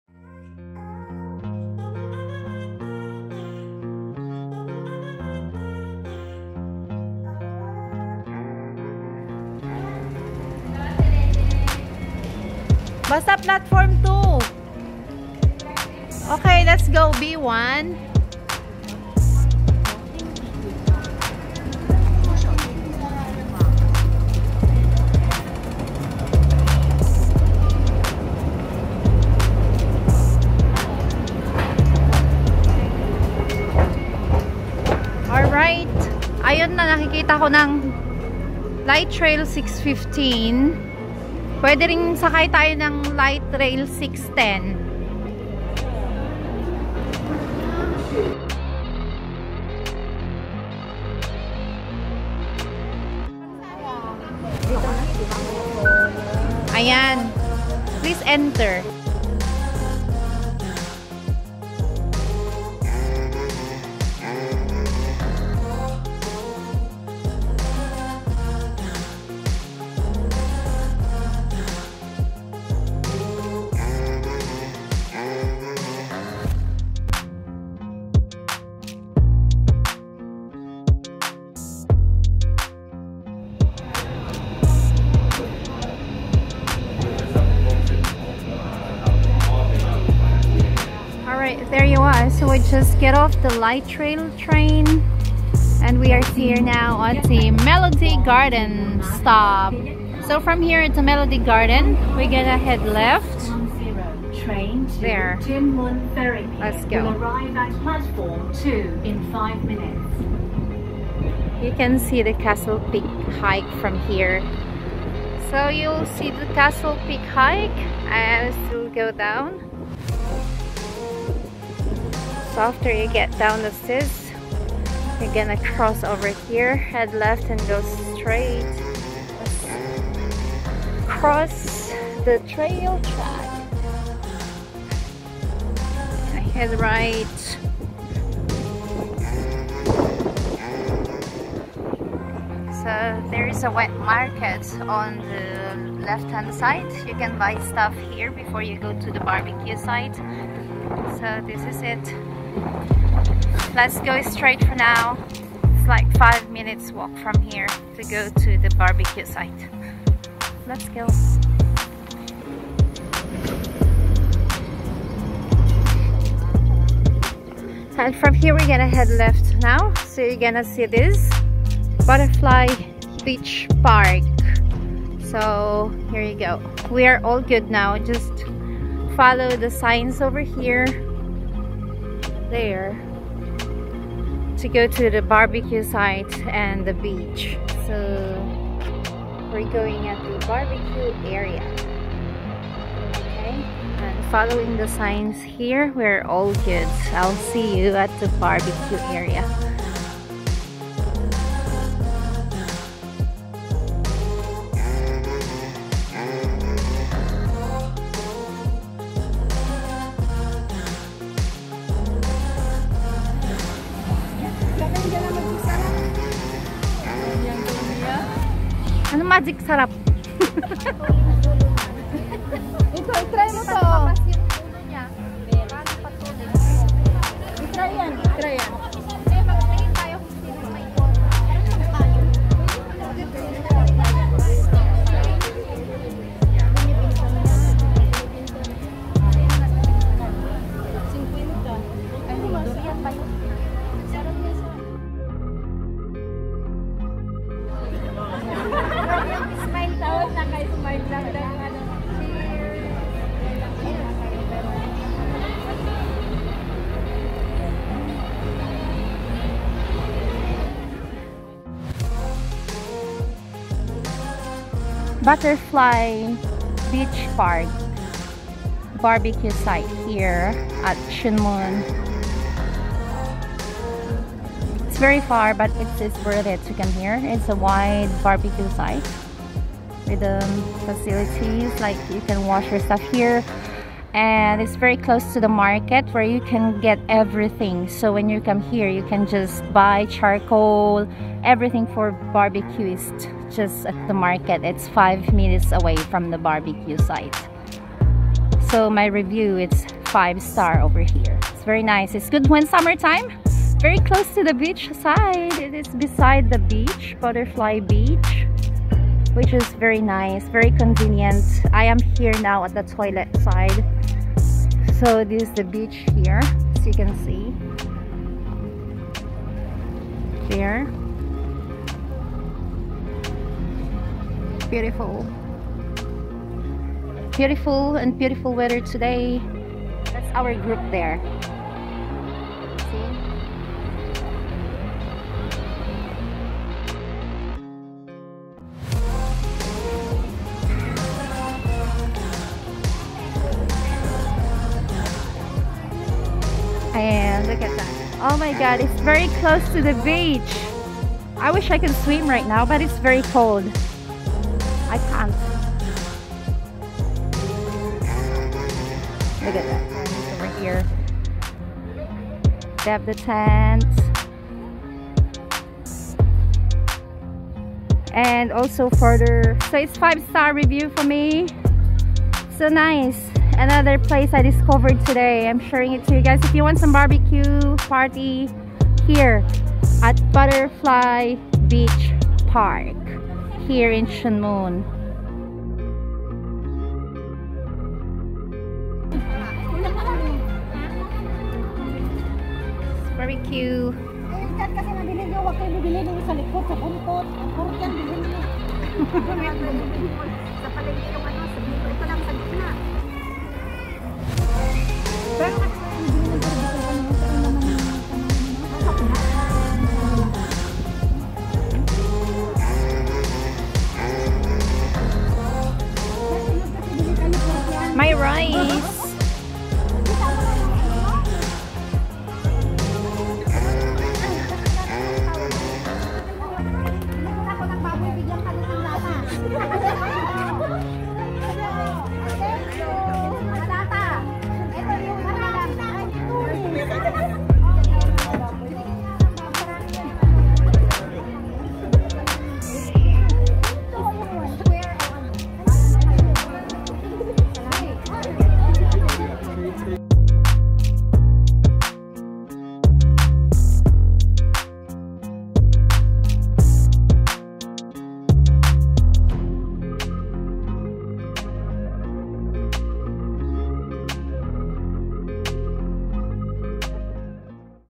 BUTTERFLY BEACH & BBQ PARK HK | HOW TO GET THERE, VIEWS, HIGHLIGHTS | SPACIOUS & VERY CONVENIENT
BUTTERFLY BEACH PARK- Located in Tuen Mun Hong Kong. It is well maintained, clean, very convenient and spacious barbeque area very close to the beach! For now, it's the only barbecue site I know here in HK where you can order things without leaving  the place you occupied for that day. Just order from the park keepers/park staffs and they will deliver the things right in your designated site.

You can order charcoal, bbq grill/net, bbq stick, food tongs, plastic gloves, paper cups/plates, tissues, aluminum foils, beverages, ice cubes, outdoor arm chair & sun umbrella rentals and you can pay by Alipay or Wechat. The barbecue area is 5 minutes away from the wet market where you can also buy meat, fish(seafoods) and other vegetables for your barbecue!

Watch the video to get HIGHLIGHTS of the place and you might want to plan for bbq with your friends, I see some people stayed there overnight with their tents as it's also a public holiday the next day. Good to know aswell.